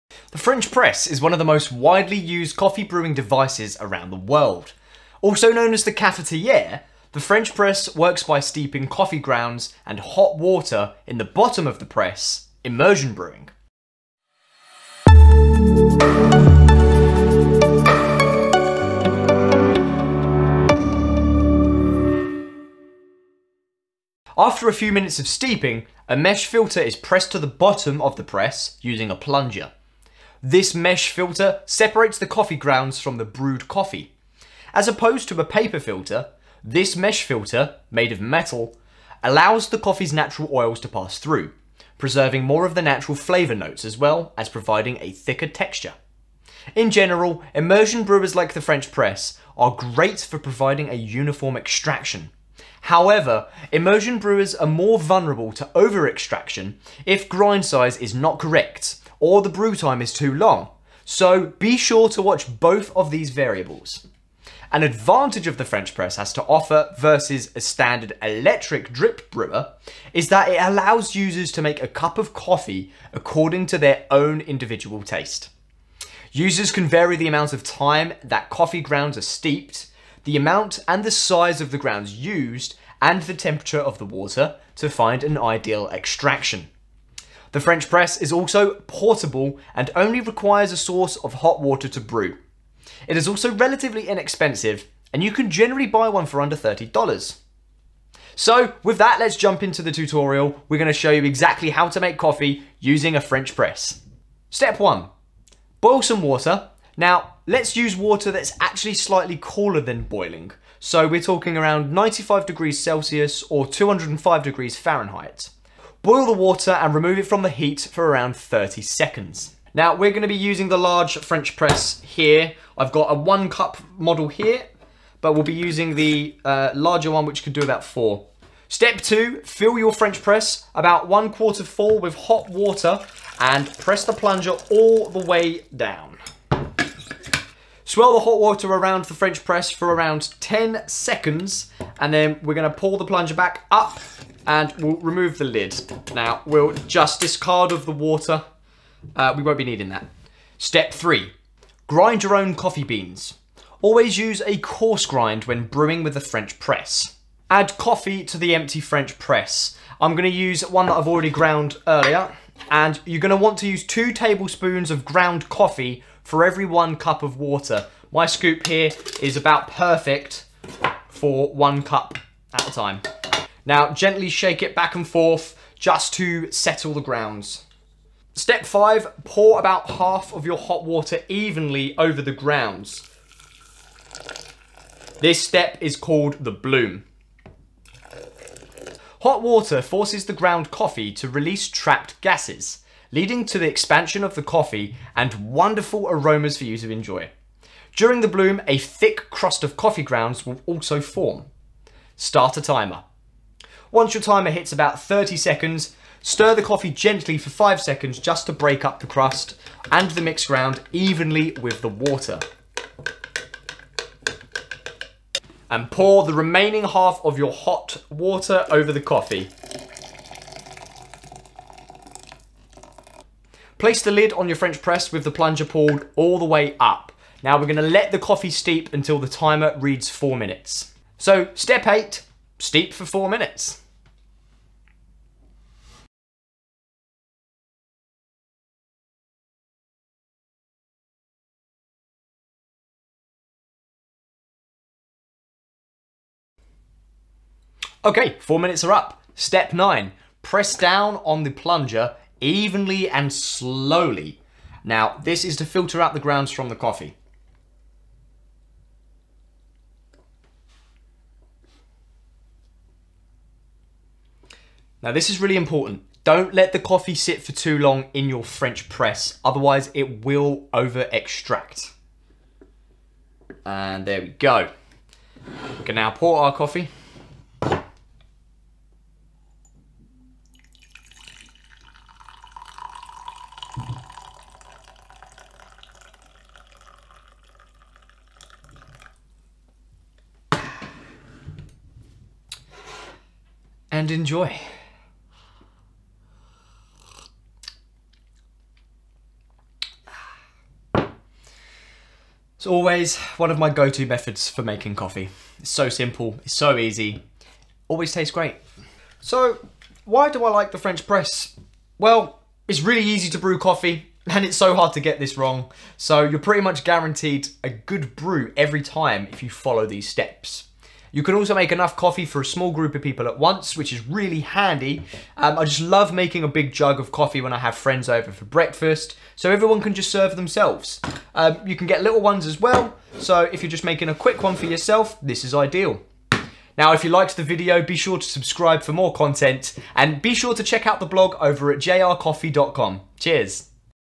How to Use a French Press (The Most AMAZING Coffee You’ve Ever Had!)
Ever see a French press and think "looks cool, but way too complicated"? Well, we've got you covered! And its a lot easier than you think!

Let us demonstrate step-by-step how to make amazing coffee in this classic coffee brewing device!